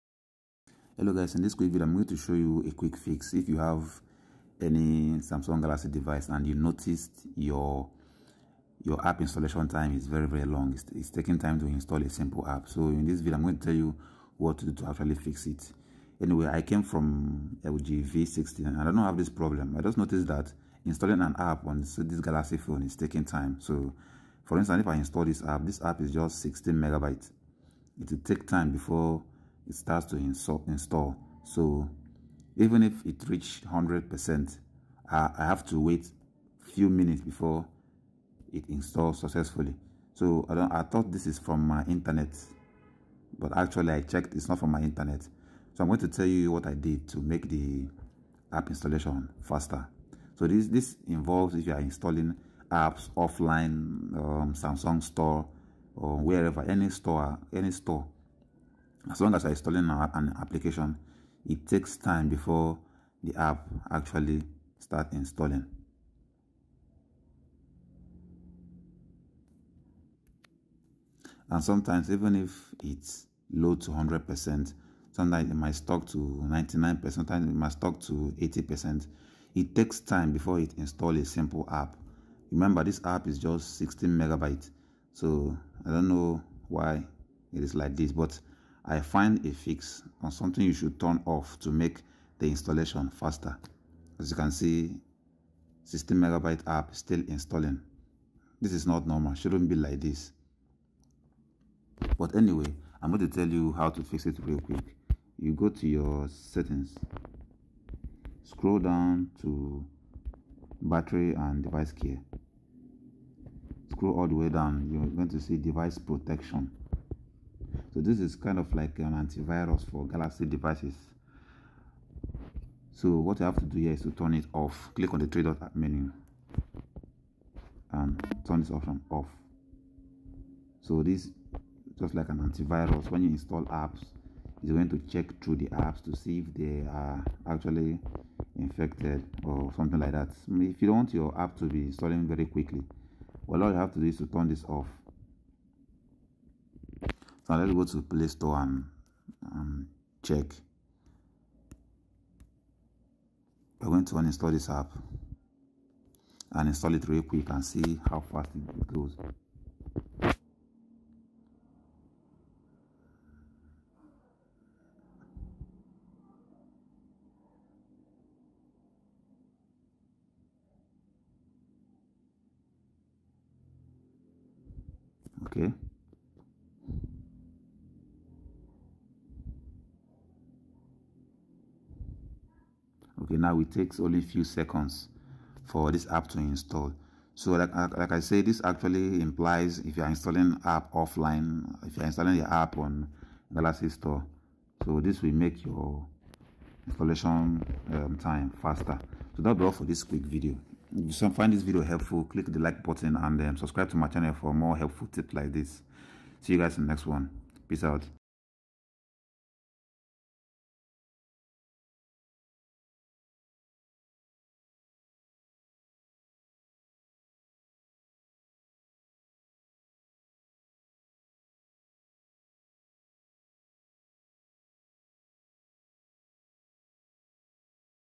Slow App Installation On OneUI? Toggle This Switch Off
Are you tired of waiting for ages for apps, updates, or software installations on your Samsung device? Say goodbye to slow installation times with our step-by-step guide, this video tutorial will walk you through proven methods to speed up the installation process, saving you valuable time and frustration I hope this video helps you, thanks for watching...